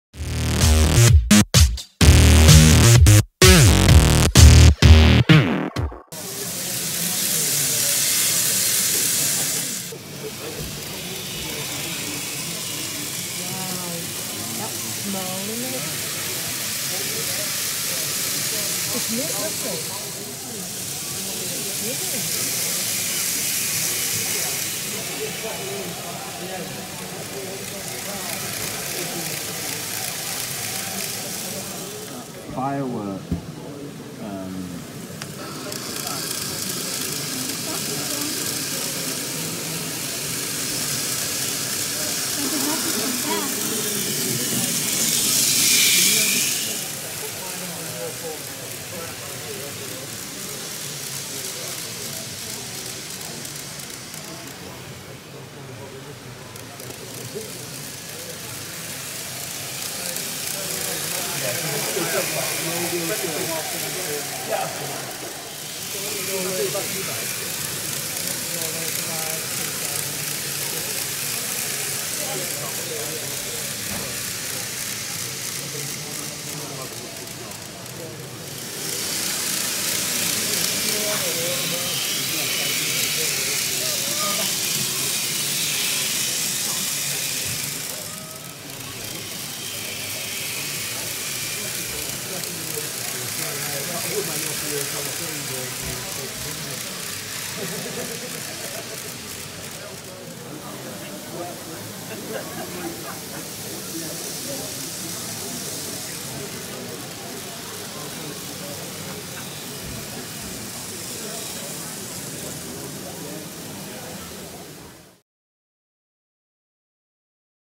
The last show for this Z gauge layout on bullet trains 10-March 2019
By the time you watch this I have already started to strip down the layout.  A new one is already almost half-done with it showing the bullet trains running by and through Japan’s mountains with real waterfalls and streams into lakes. Viaducts and so one are some of the additional features at our next show.  A small Shorty diorama is added on the end as it hid all the train controls.